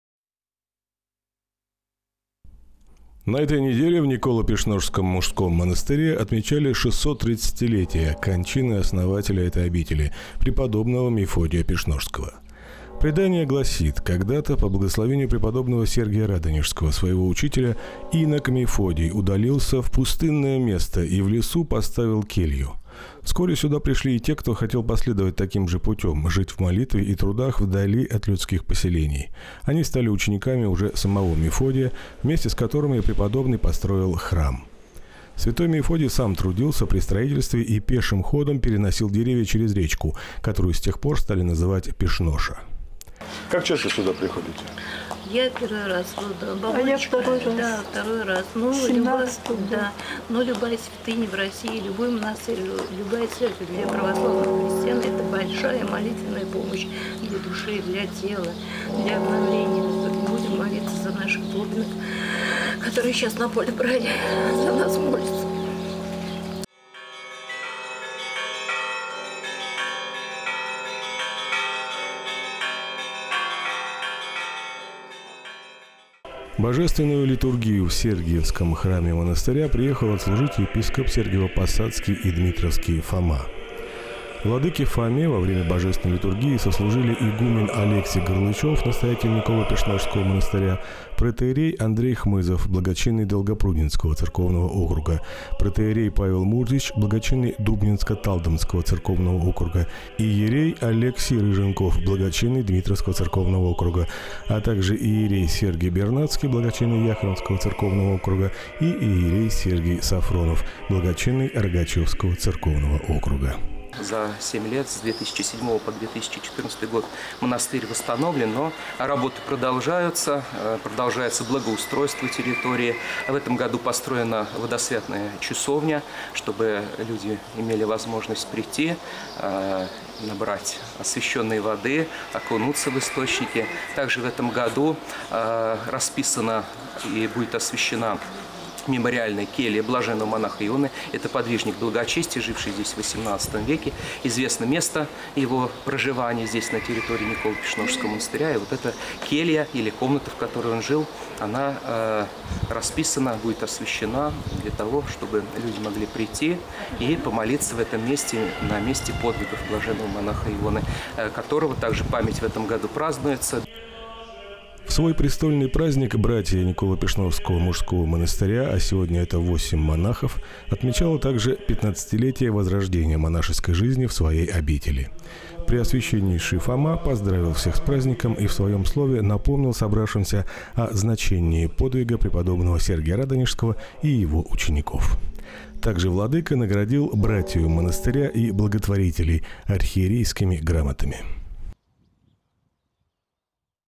Торжественное богослужение в честь основателя Николо-Пешношского монастыря, преподобного Мефодия
На днях Николо-Пешношский мужской монастырь посетил епископ Сергиево-Посадский и Дмитровский Фома, который возглавил торжественное богослужение в честь основателя этой обители, преподобного Мефодия Пешношского